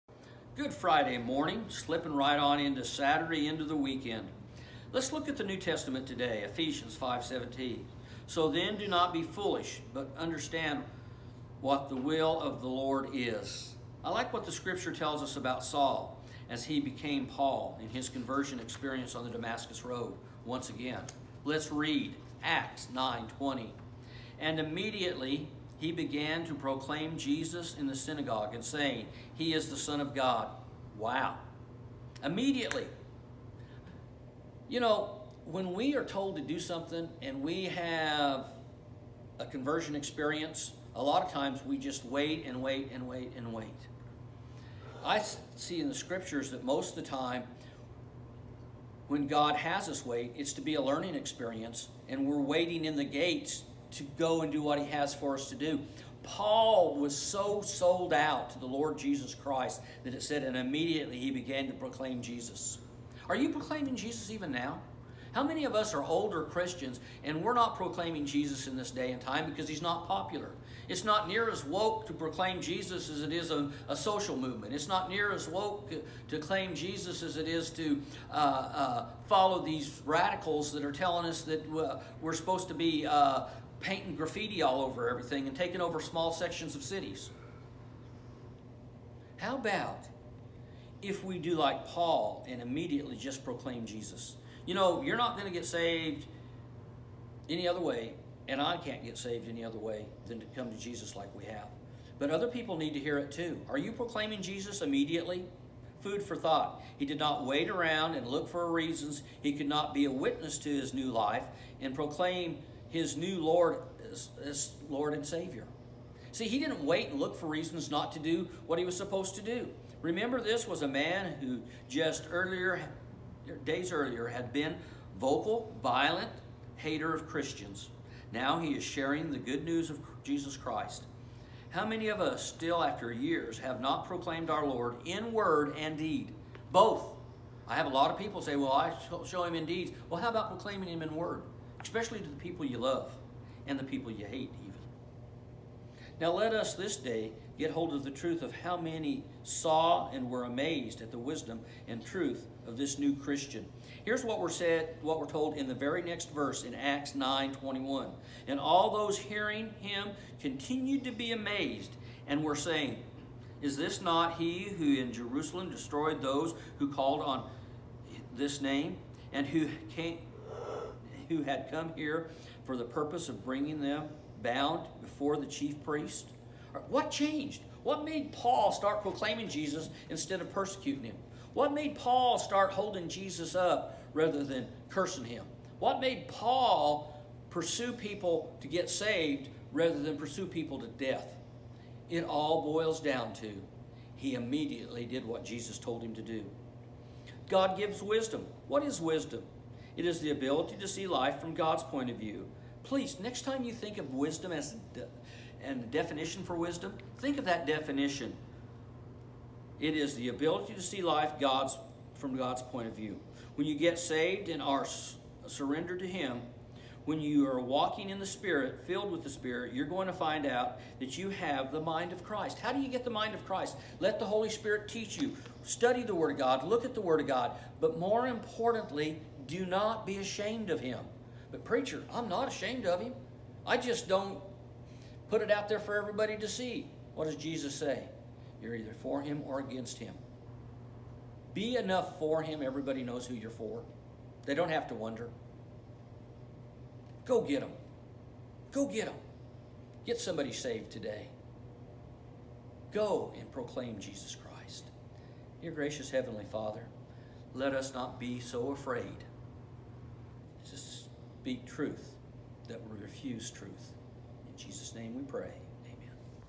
Daily Message - 6/26
Here’s your daily message to help you through your day.

Pastor: Bro. Brett Castle
East Baptist Church - Denison, TX

You can send your tithe through:
or go to ebchurch.com and click on the "Donate" button at the bottom of the website.